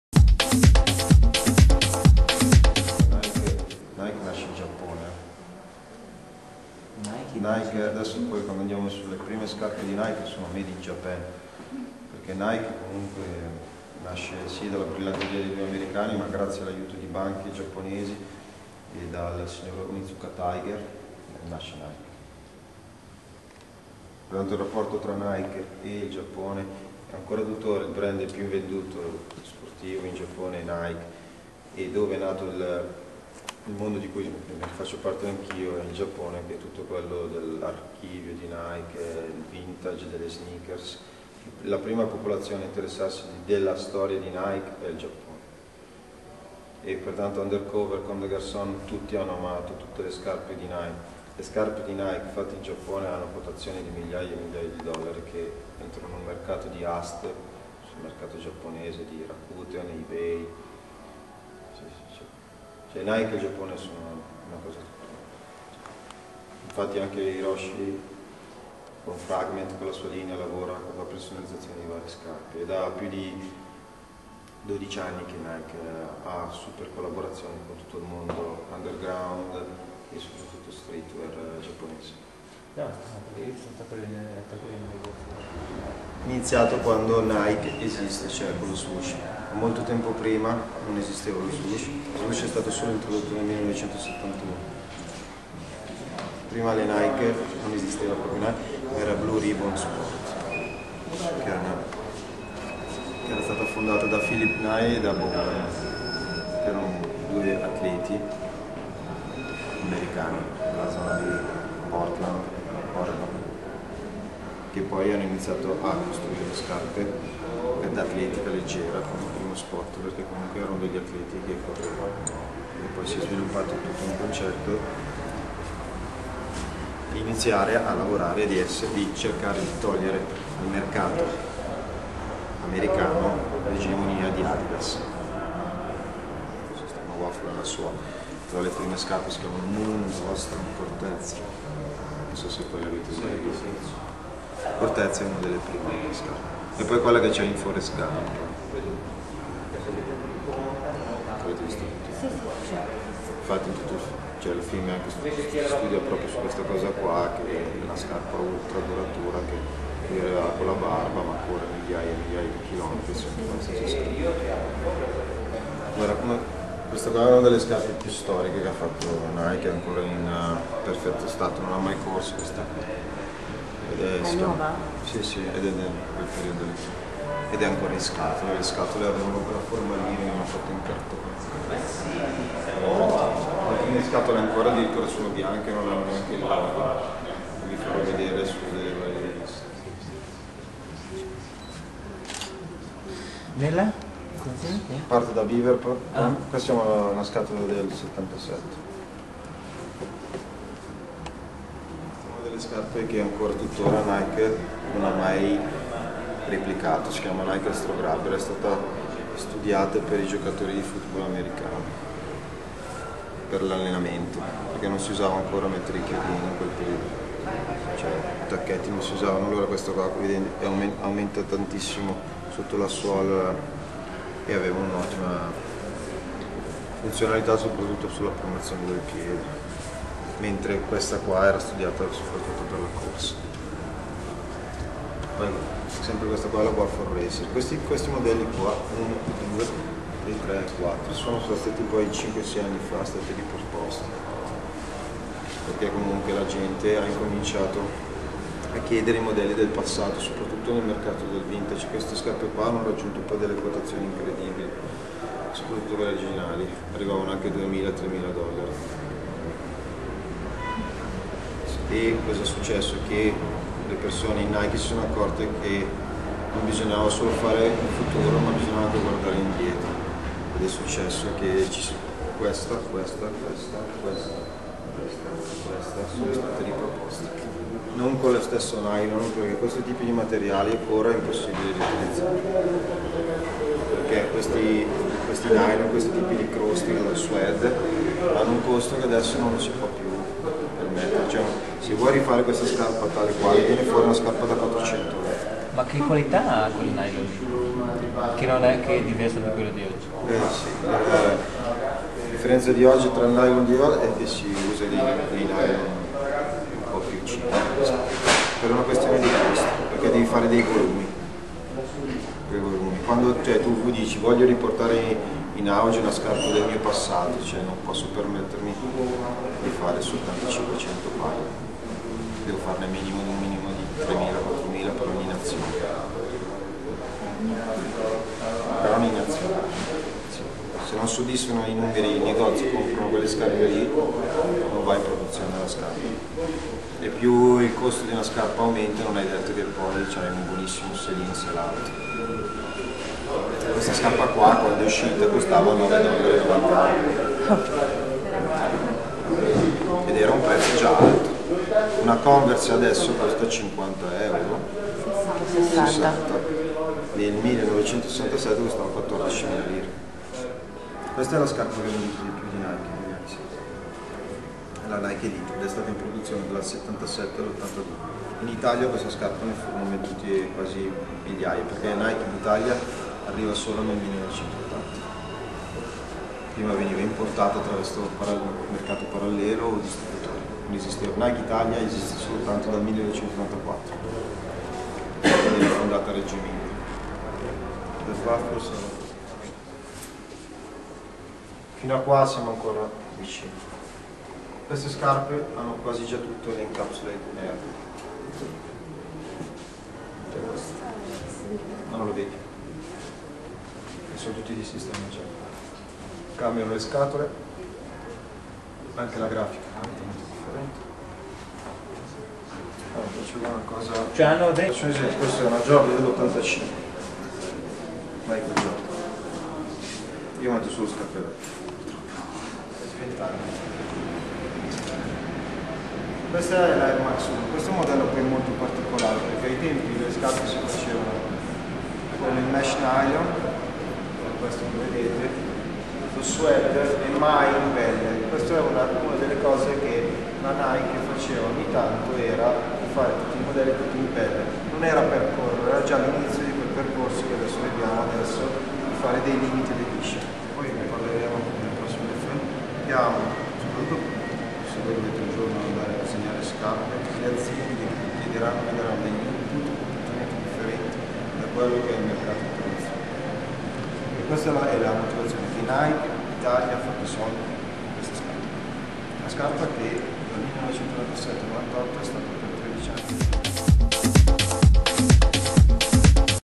Marco Colombo: la storia delle sneakers NIKE
Marco Colombo @ Nike Stadium ci parla della storia delle sneakers NIKE